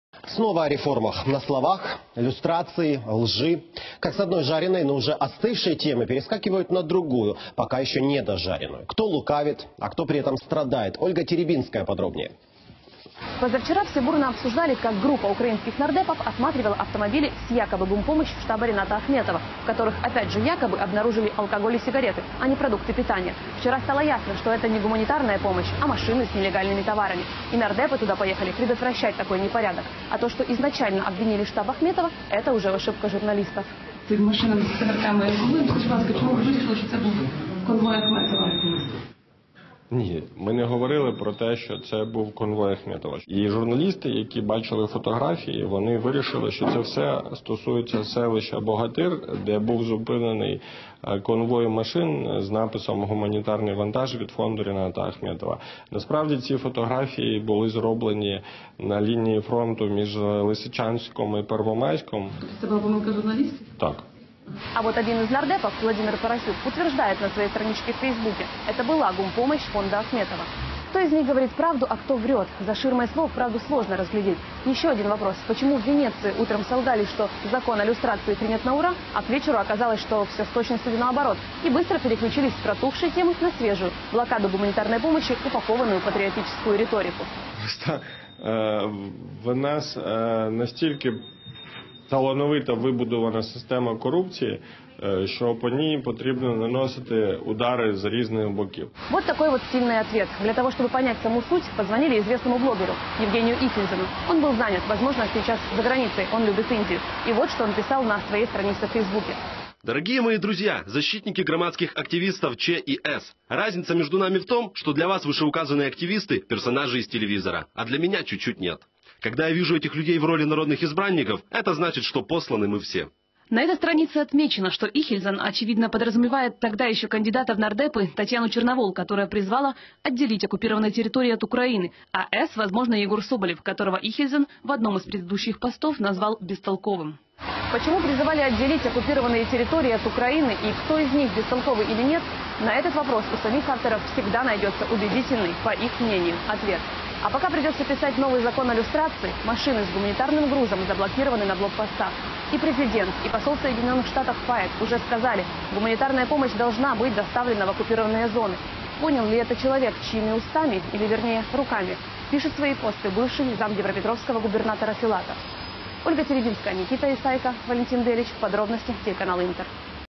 Соболев открещивается от контрабанды Ахметова
Народный депутат Егор Соболев заявил, что фотографии с алкоголем и сигаретами не принадлежат гумконвою Рината Ахметова. А вот один из нардепов, Владимир Парасюк, утверждает обратное.